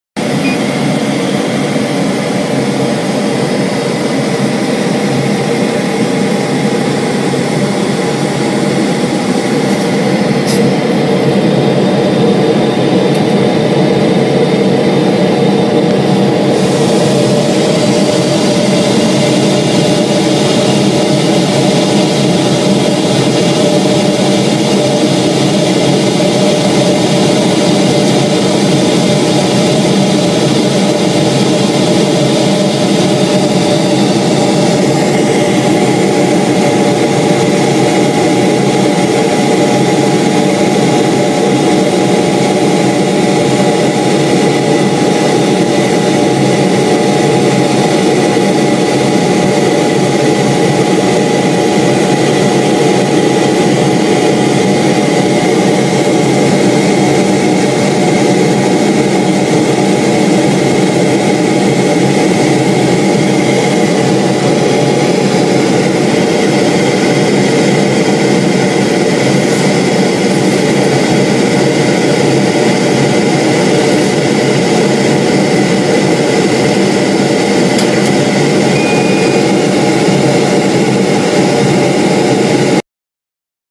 D1210 high power laser cutting machine cut the plexiglass 00
Multiplicate data transmission mode

Optional laser tube(100W,120W,150W)

Laser die-cut/acrylic cutting machine

Can cut max 1.5mm metal sheet

In order to realize the processing need, this laser device has used the imported accuracy mechanical system and the high performance control system which constitute complete machine. This equipment operation is steady, precise and easy to handle, the friendly man-machine conversation contact surface may realize the high quality processing effect for various trades and occupations.